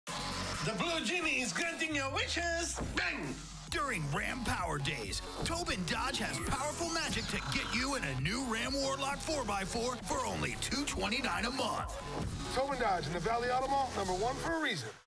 Towbin Dodge | Lease Ram Warlock 4x4 for $229 Per Month
The Blue Genie is grating your wishes!  During Ram Power Days lease a new 2022 Ram Warlock 4x4 for only $299 a month.  Visit us today!

Offer expires 09/30/2022